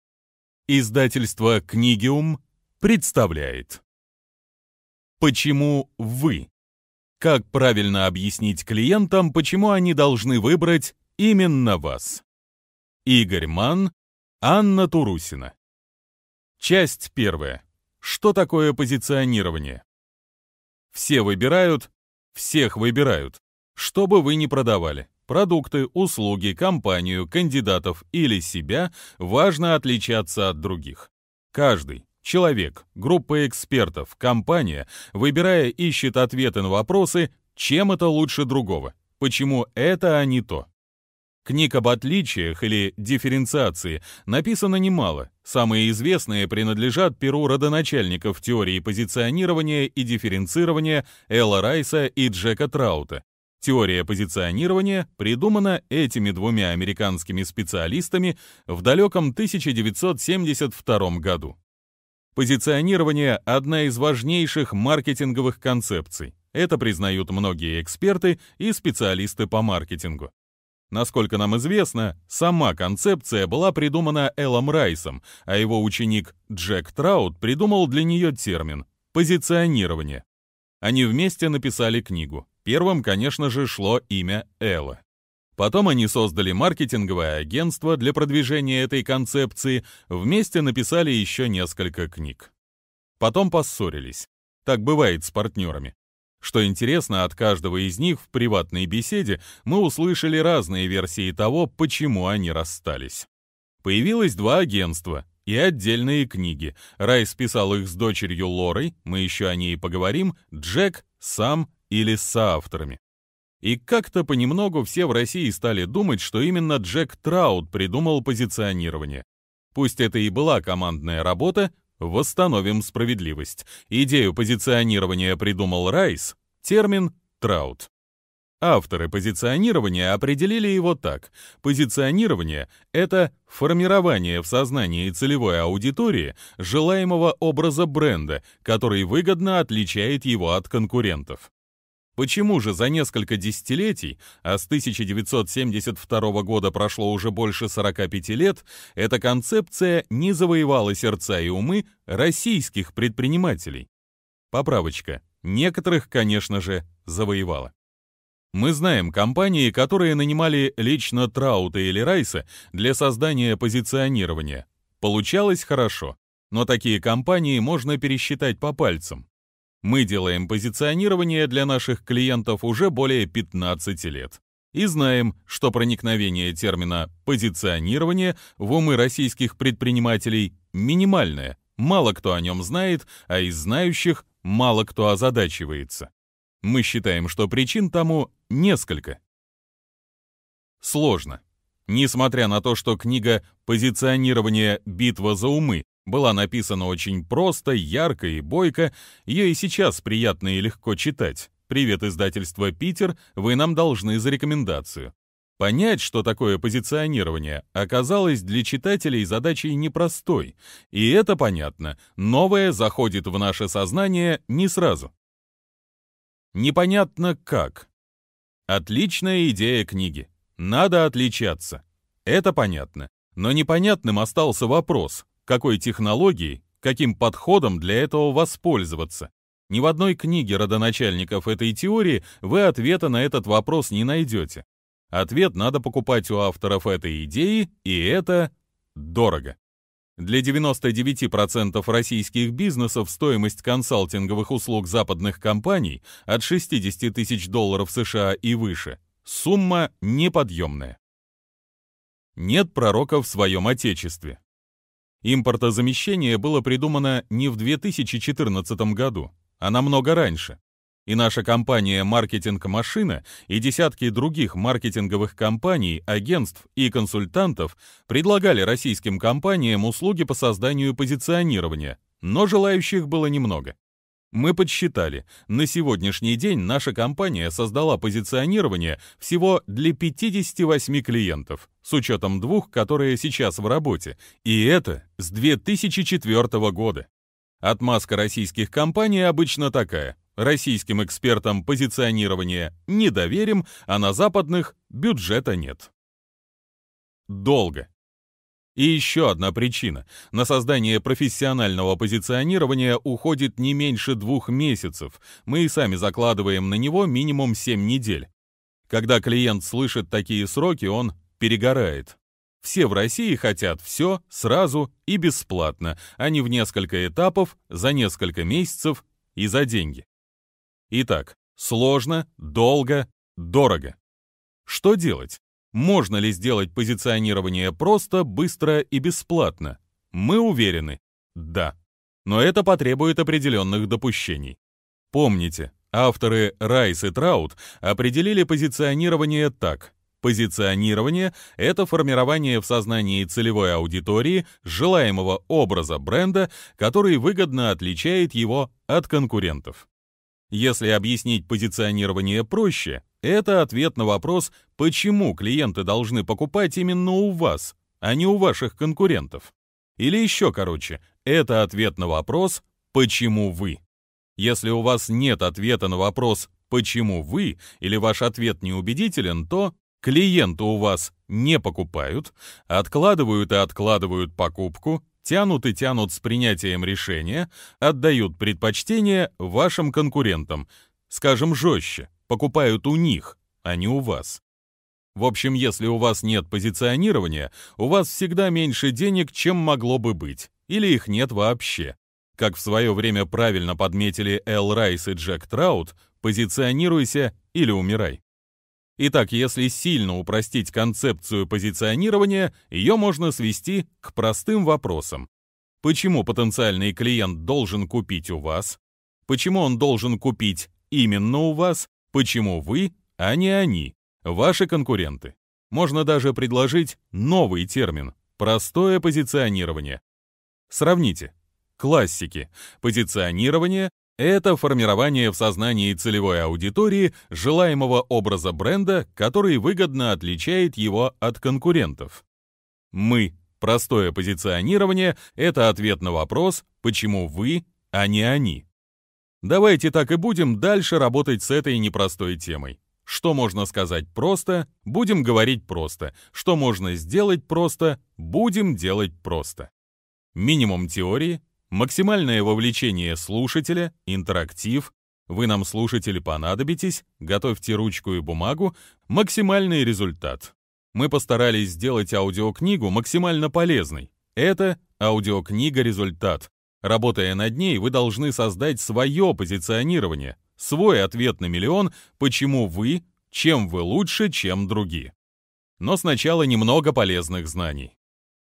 Игорь Манн – Почему вы? Как правильно объяснить клиентам, почему они должны выбрать именно вас.
Аудиокнига Почему вы? Как правильно объяснить клиентам, почему они должны выбрать именно вас Игоря Манна – слушать онлайн.
Авторы: Игорь Манн, Анна Турусина.
Жанры: клиентский сервис, привлечение клиентов, стратегия маркетинга, управление маркетингом, управление продажами
Дата выхода на ЛитРес: 16 февраля 2021
Дата написания: 2020
Чтец: Павел Константиновский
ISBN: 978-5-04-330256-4

Описание: Предложений много, а клиент один. По крайней мере, так видит ситуацию сам клиент. Он выбирает, у кого купить. У вас? А почему у вас, собственно? Почему вы – лучший выбор для него? От того, насколько грамотно вы ответите на этот вопрос, зависит… Да в общем-то, весь ваш бизнес и зависит. Если вы не сумеете это обосновать – прогорите. Если ответите убедительно – преуспеете. Здесь все о том, как отвечать правильно.